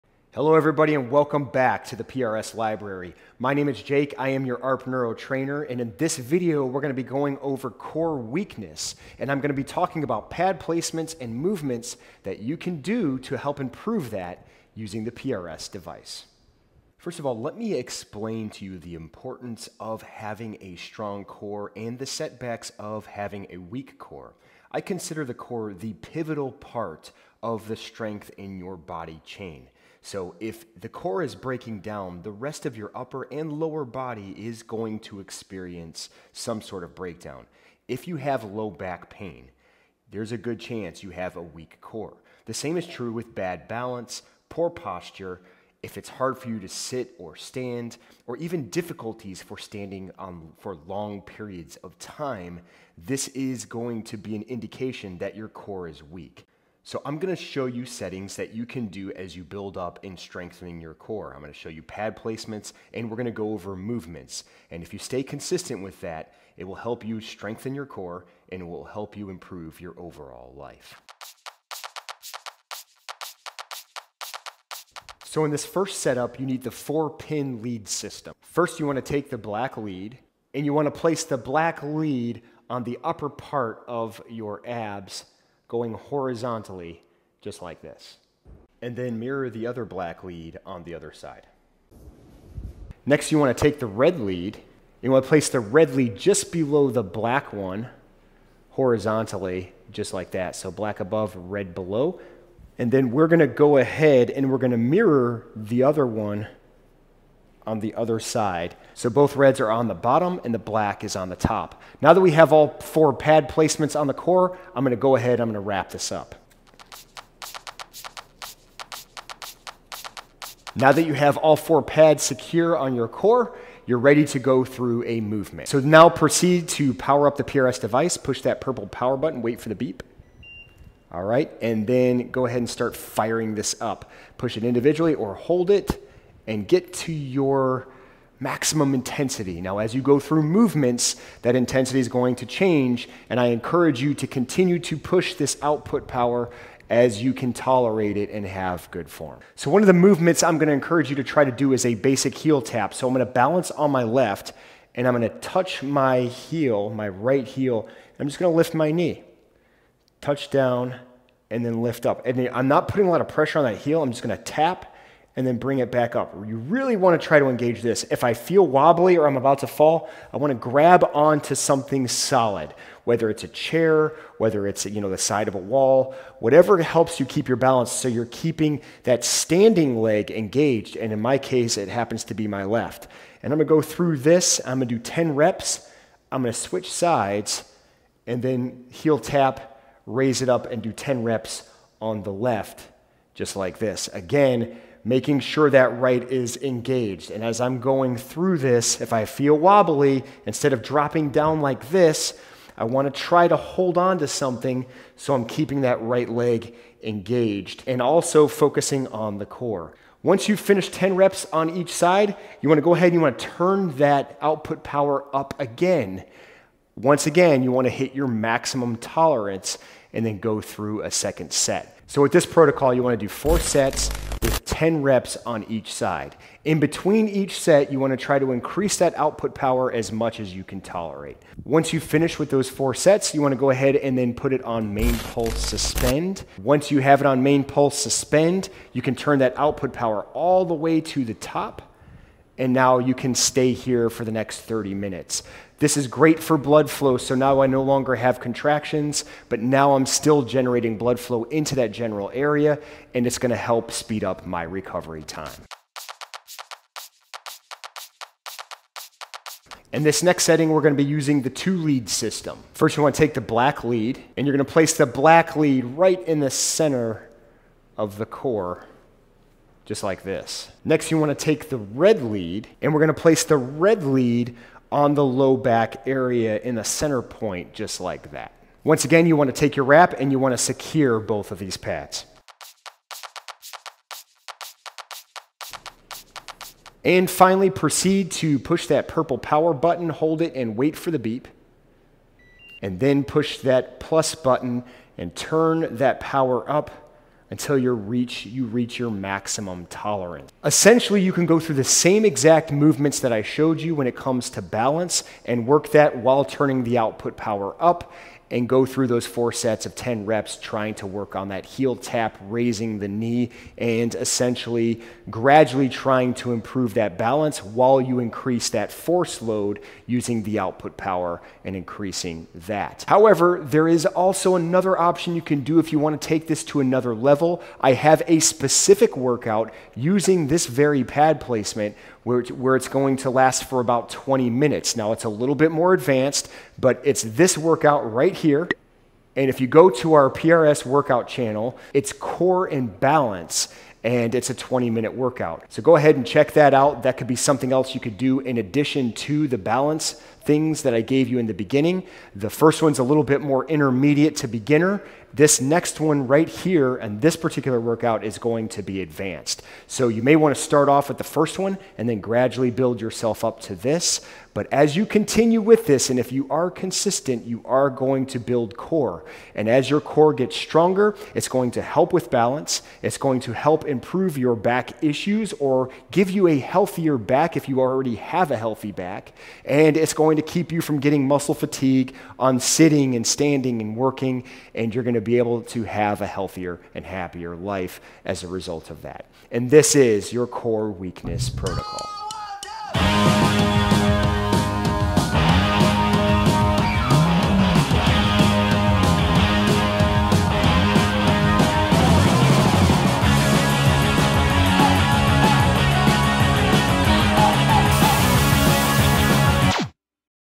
How to Use Direct Current: Transform Core Weakness Into Power!
Core weakness can result in lower back pain, poor balance, poor posture, and difficulty standing for long periods.

This video shows how to use the ARPwave PRS to build core strength.

0:00 What is Core Weakness
1:15 Pad Placement Option #1
2:19 Movement Exercises
4:22 Pad Placement Option #2

Direct Current Neuro Therapy is not to be used by women who are pregnant. If you have an implanted electrical device, cancer, or a history of blood clots, please consult your physician before use.